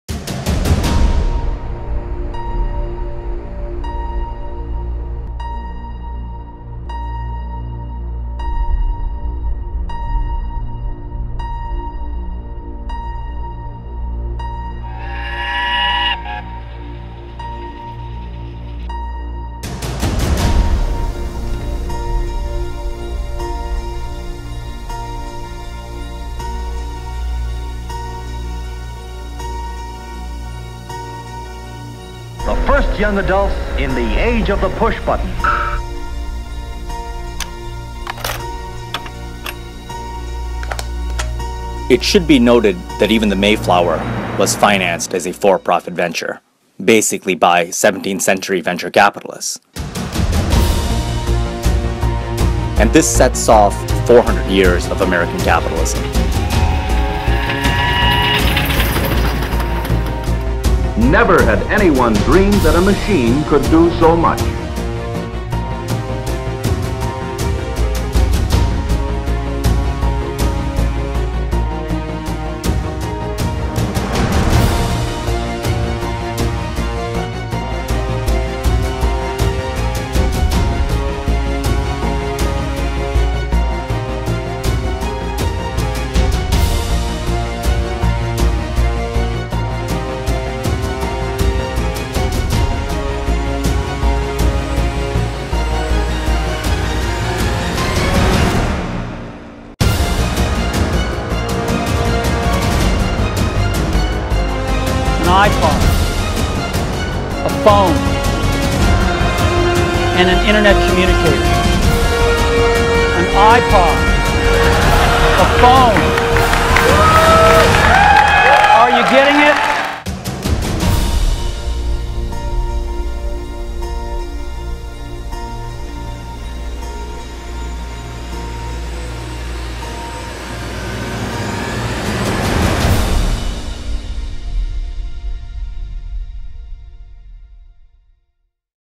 Americana Sizzle
Based on the upcoming book from Bhu Srinivasan, AMERICANA - The History of American Capitalism, this exclusive sizzle reveals how the narrative sweep of America's economic system has touched shaped and defined its culture, democracy, and identity.